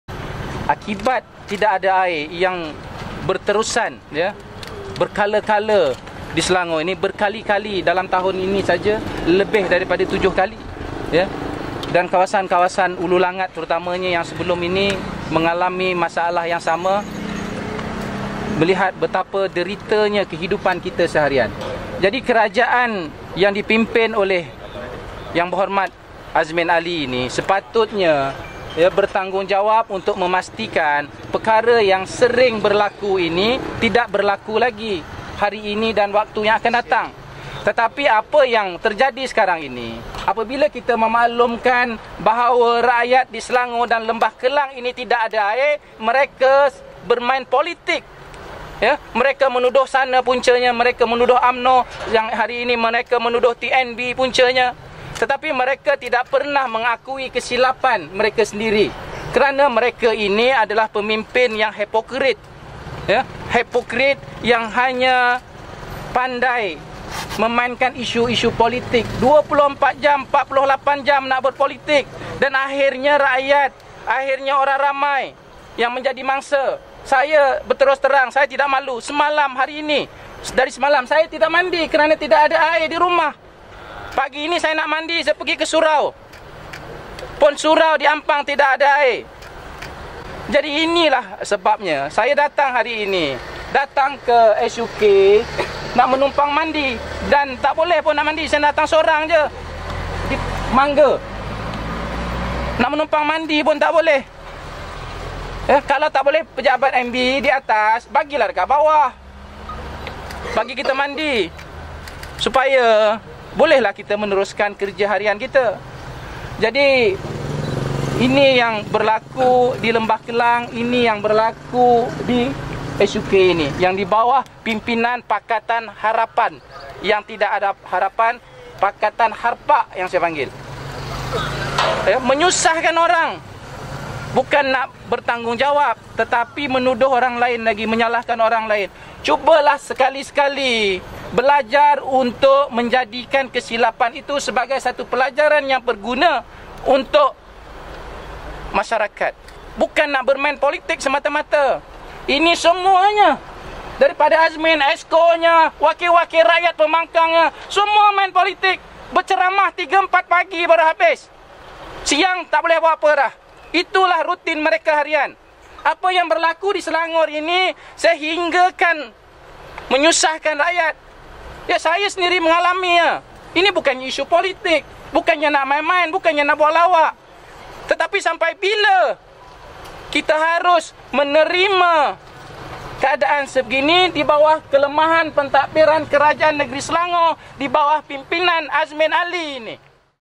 Krisis Air : Jangan Main Politik,Pentingkan Kebajikan Rakyat Selangor
Pengerusi Gabungan NGO Malaysia,Dato' Sri Jamal Md Yunos meminta agar Menteri Besar Selangor,Datuk Seri Mohamed Azmin Ali agar tidak mempolitikkan krisis air yang berlaku di negeri selangor.beliau meminta Kerajaan Negeri Mementingkan Kebajikan Rakyat Selangor